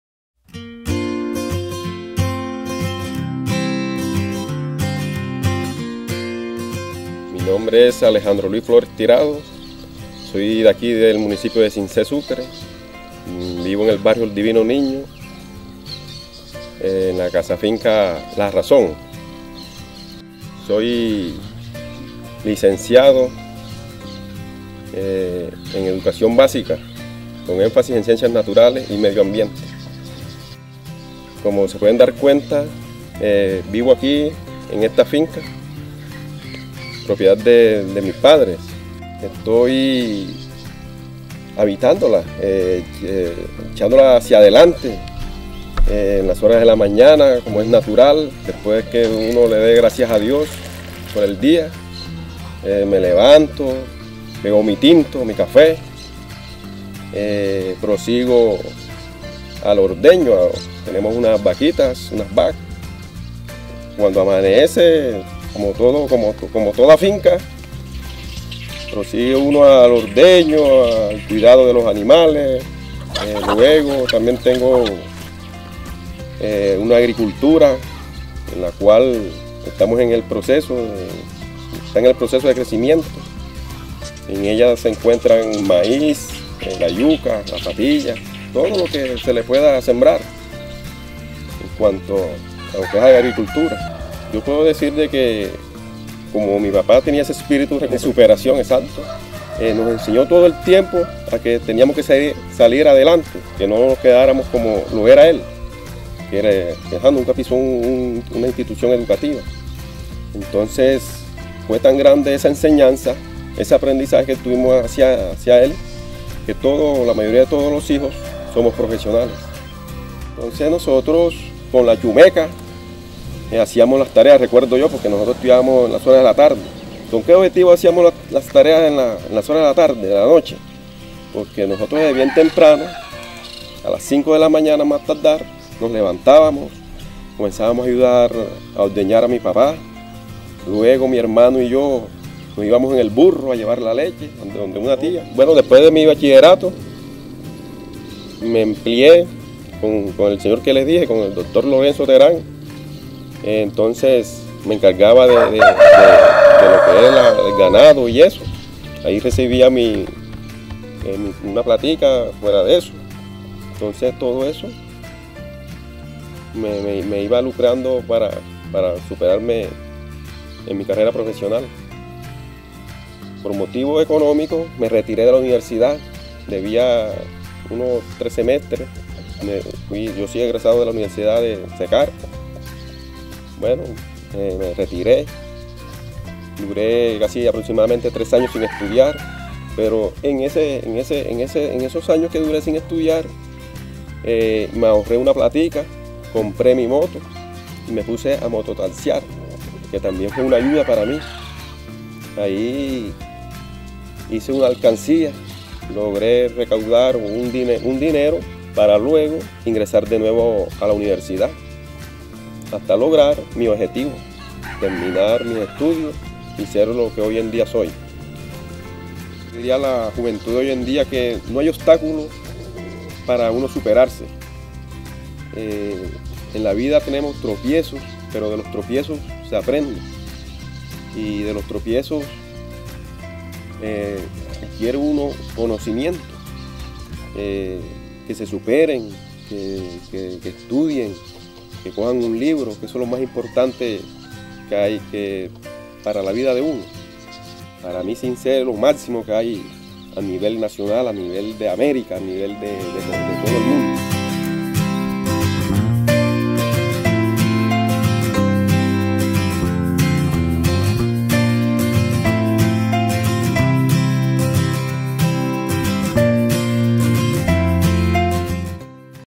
Alejandro Luis Flòrez Tirado
Alejandro Flórez es un sinceano ejemplo de superación, que pese a las dificultades económicas pudo graduarse como Licenciado de Ciencias Naturales y Medio Ambiente. Actualmente vive en una pequeña finca a las afueras del municipio donde también realiza labores del campo, algo que aprendió desde que era un pequeño. ¡Conócelo!